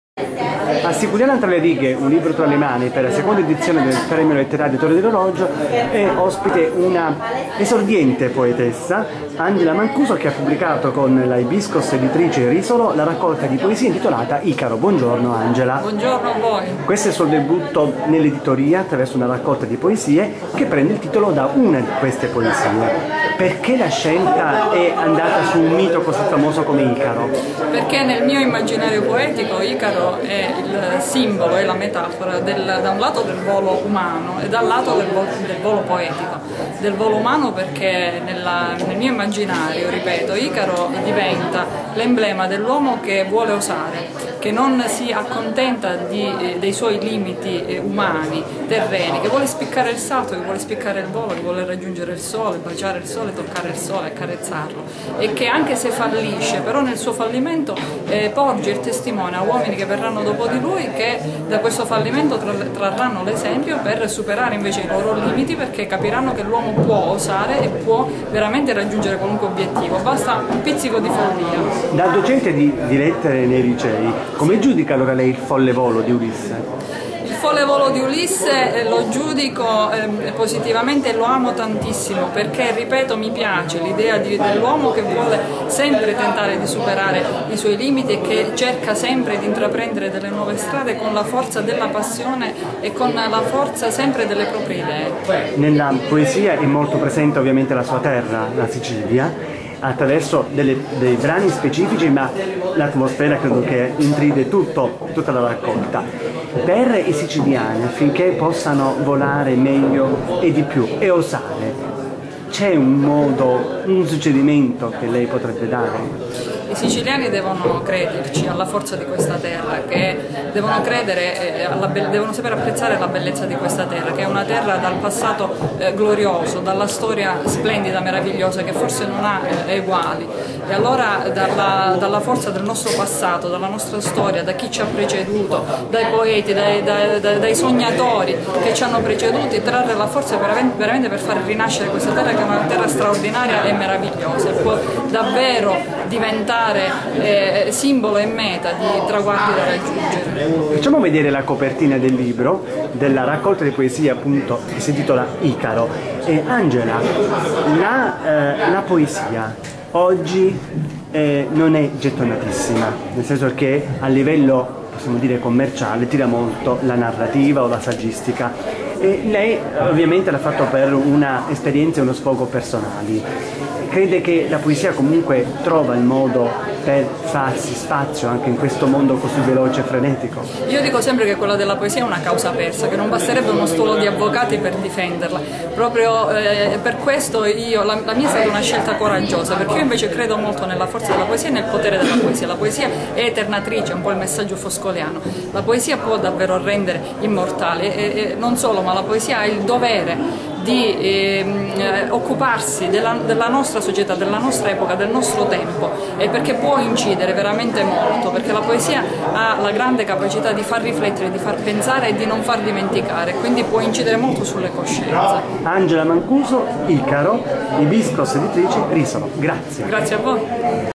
"Icaro", poesie di Angela Mancuso: "L'uomo può raggiungere qualsiasi obiettivo"
Un viaggio che porta il lettore attraverso l'amore per la Sicilia e figure mitologiche i cui significati e messaggi sono validi ancora oggi è quello che la scrittrice Angela Mancuso compie e invita a compiere con le poesie raccolte in "Icaro" (Ibiskos Editrice Risolo, € 10,00), che è anche il titolo di una delle liriche presenti. Un personaggio che è metafora da un lato del volo umano, dall'altro del volo poetico, ha riferito la stessa autrice a Fattitaliani, che l'ha intervistata a Siculiana durante un incontro letterario svoltosi nell'ambito della Fiera del Libro e della seconda edizione del premio letterario "Torre dell'Orologio".